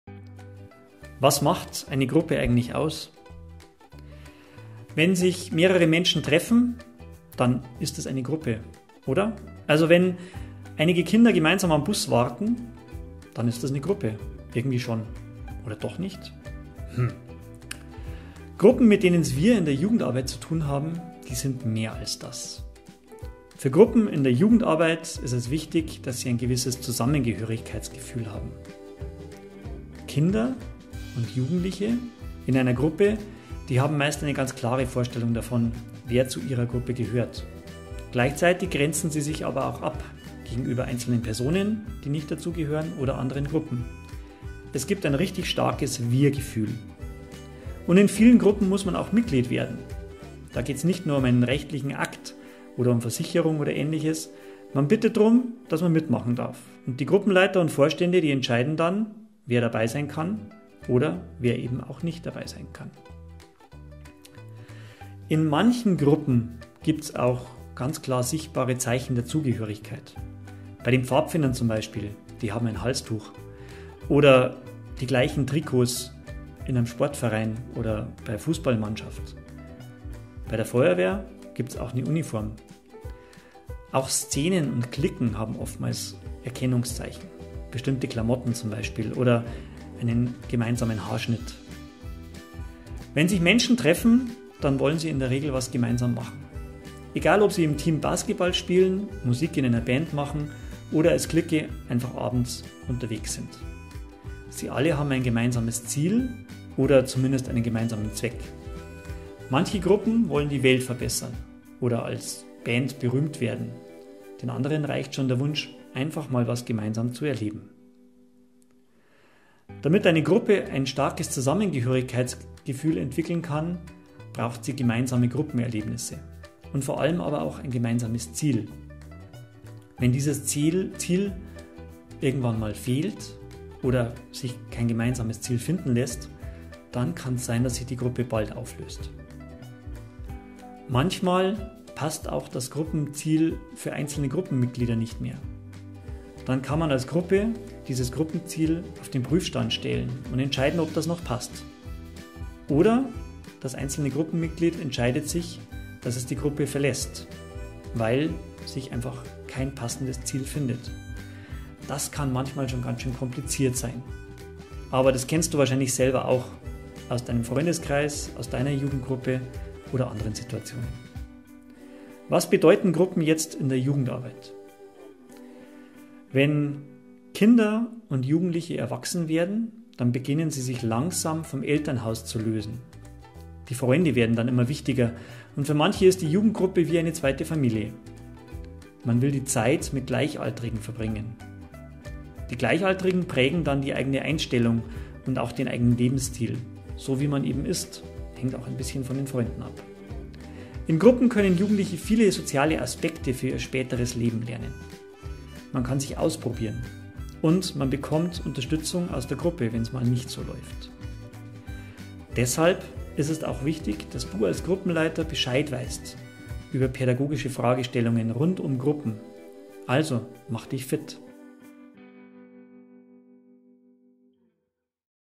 Die Gruppe in der Jugendarbeit * juleica Ausbildung * Die Gruppe
Einstieg in die Gruppenpädagogik
juleica Ausbildung
0:00 Was macht eine Gruppe aus?
1:32 Wie funktionieren Gruppen?
2:50 Bedeutung von Gruppen in der Jugendarbeit

Infos zur Gruppenpädagogik gibt es zum Nachlesen im juleica Handbuch
Kapitel 4 Die Gruppe
in enger Anlehnung an Juleica - Handbuch für Jugendleiterinnen und Jugendleiter